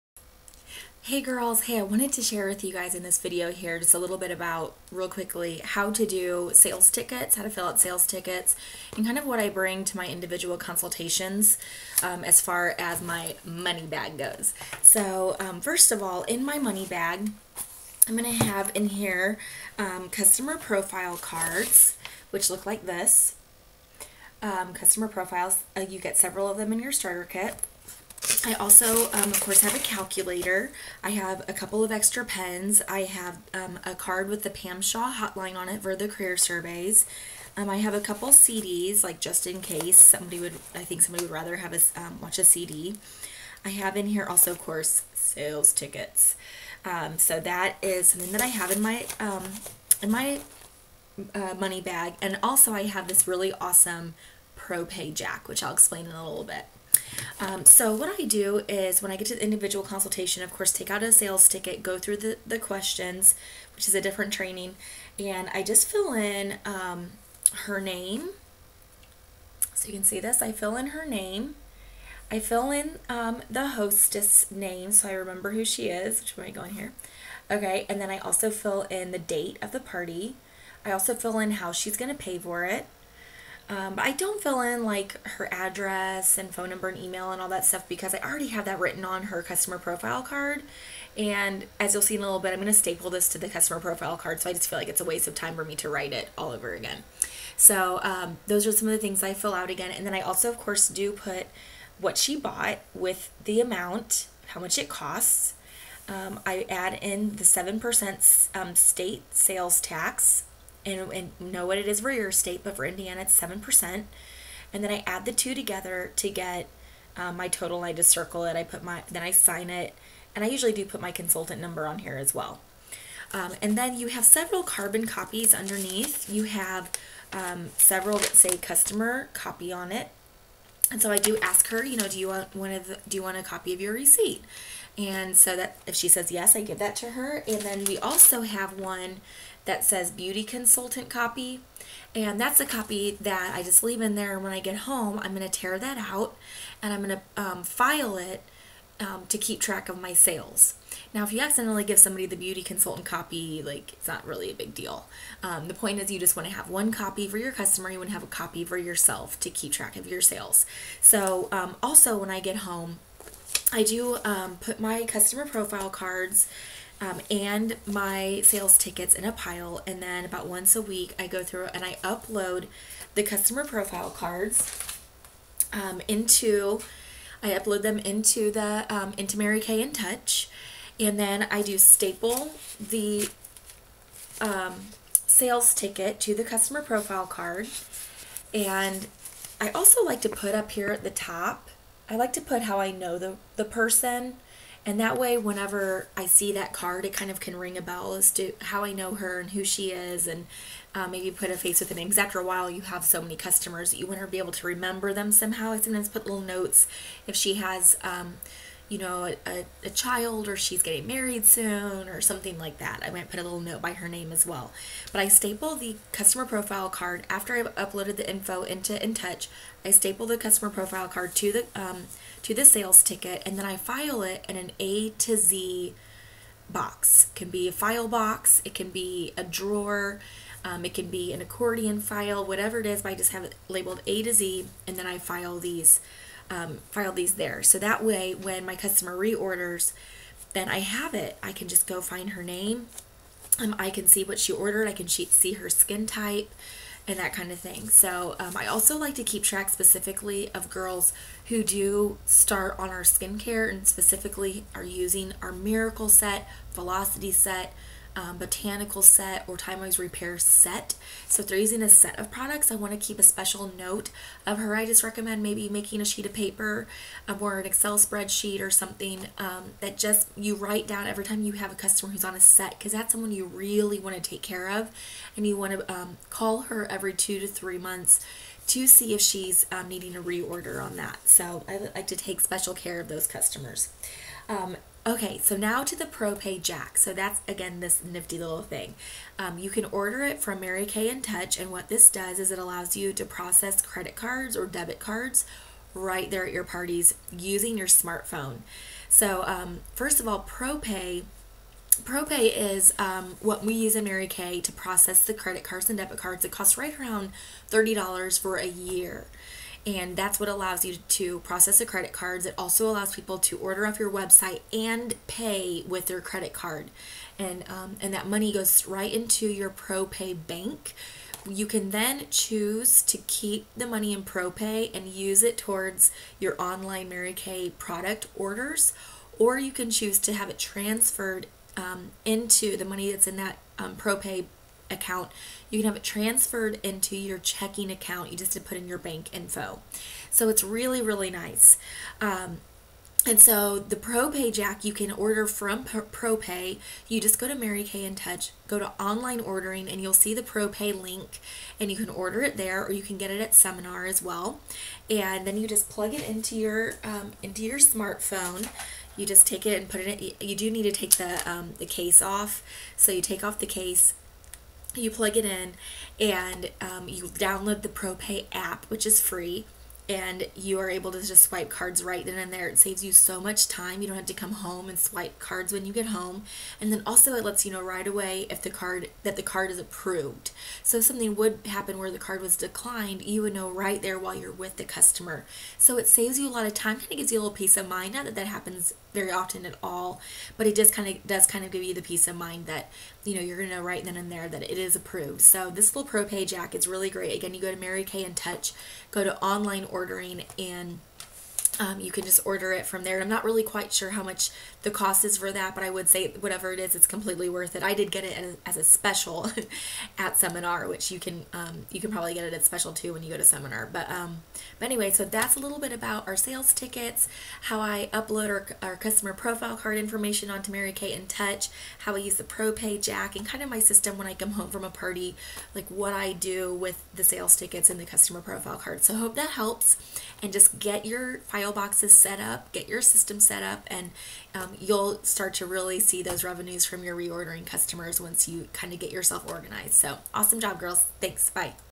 How To: Customer Profiles, sales tickets, propay
I walk you through how to organize and fill out Customer Profile Cards, Sales tickets, and use Propay to process credit cards!